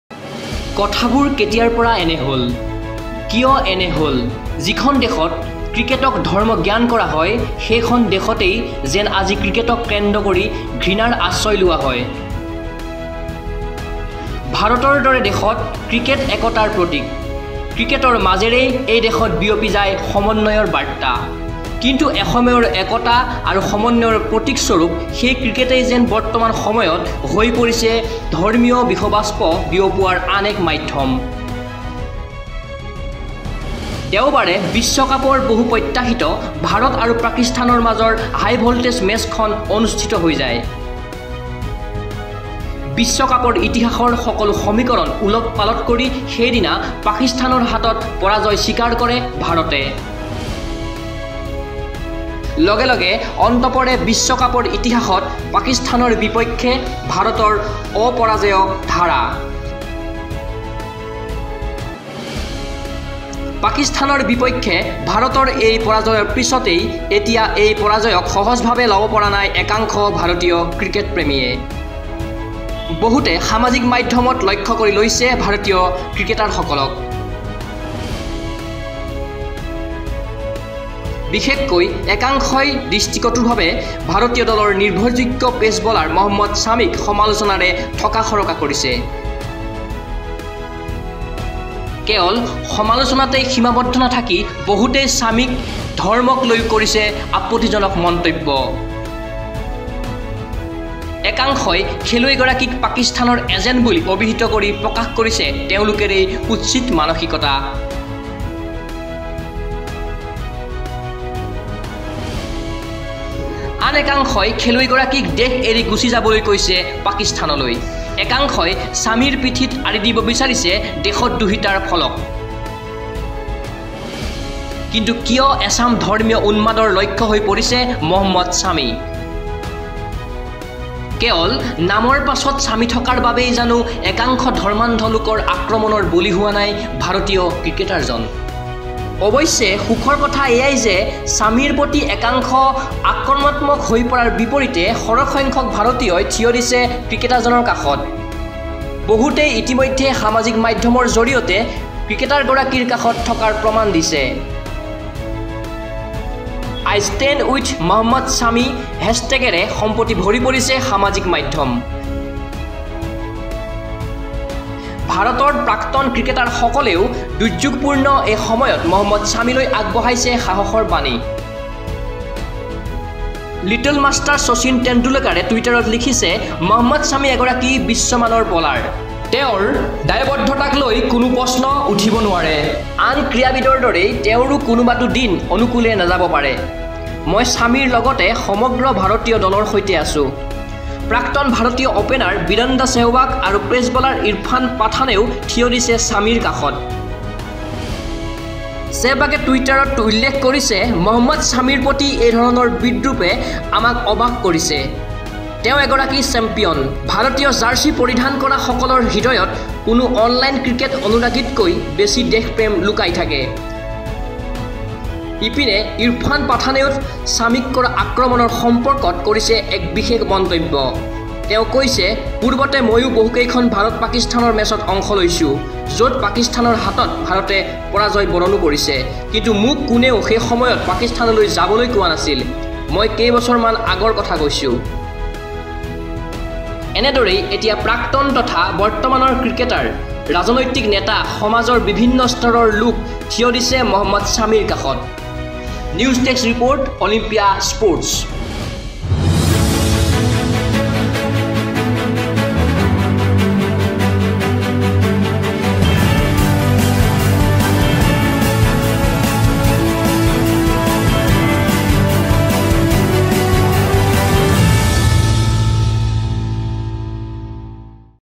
India backs Mohammad Shami | সামাজিক মাধ্যমত সমালোচনাৰ সন্মুখীন মহম্মদ ছামি
■ সামাজিক মাধ্যমত সমালোচনাৰ সন্মুখীন মহম্মদ ছামি
■ বিশ্বকাপৰ ব্যৰ্থতাৰ পিছতেই আক্ৰমণৰ লক্ষ্য হৈ পৰিছে ছামি
■ ছামিৰ কাষত শচীন, চেহৱাগ,পাথানকে ধৰি অন্যান্য ক্ৰিকেটাৰ